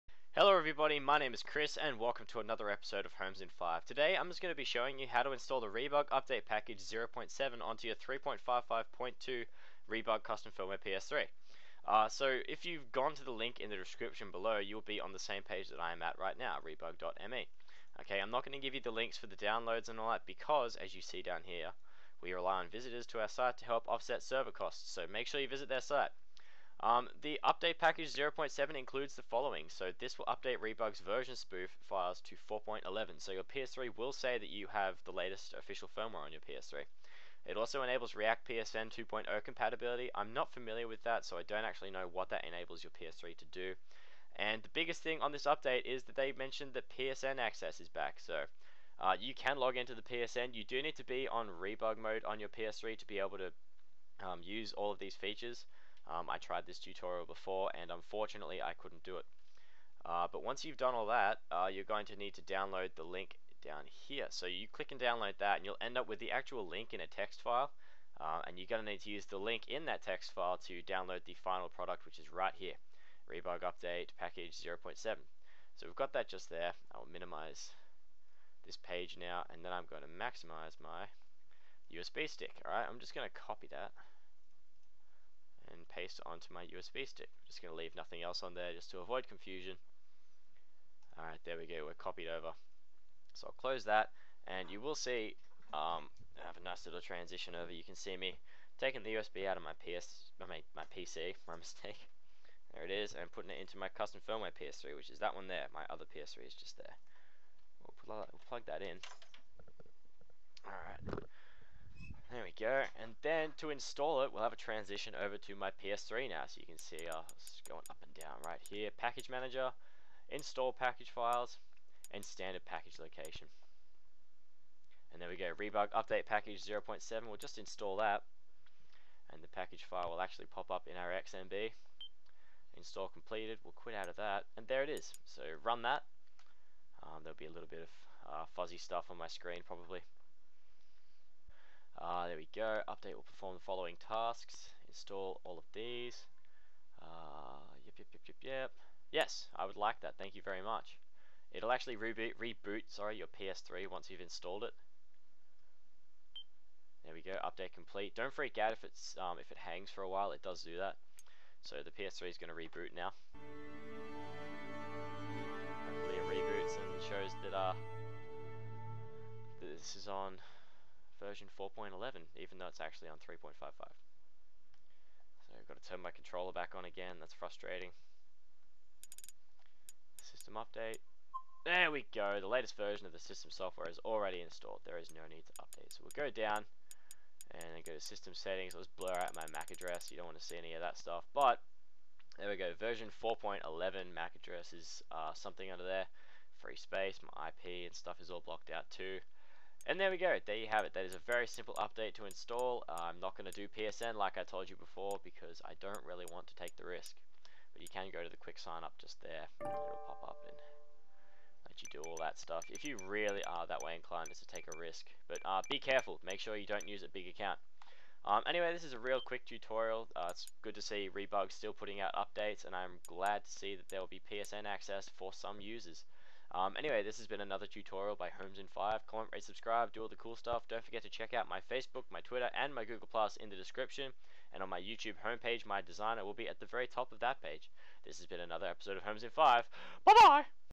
How To Install Rebug Update Package 0.7 (3.55.2 CFW)
Hope you enjoy the video!  Good news for those on Rebug, which is personally the best CFW out right now.

Yes, for the time being you CAN get online with this CFW/update.  It won't last long though, so use it while it's not blocked.